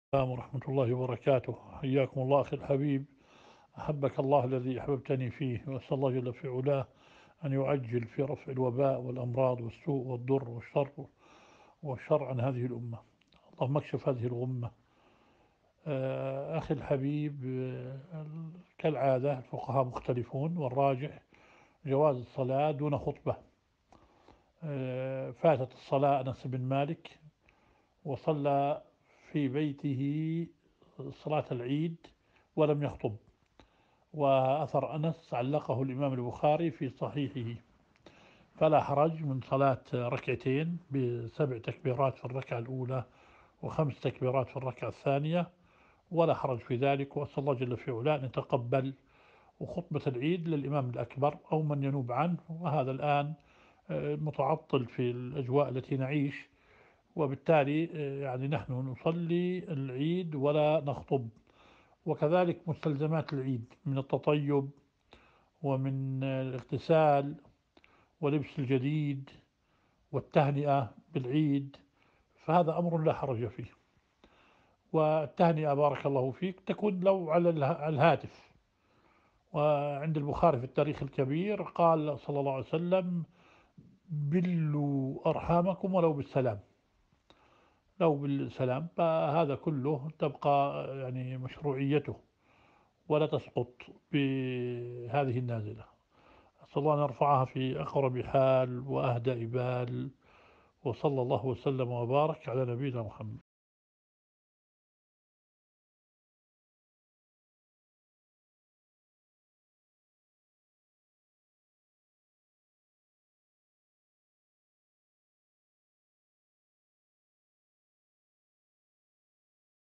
Est-il permis d'accomplir la prière de l'Aid chez soi si la mosquée est fermée? Sheykh Mashour Hasan
Est-il possible d'accomplir la prière de l'Aid à la maison si la mosquée est fermée ? Sheykh Mashour Hasan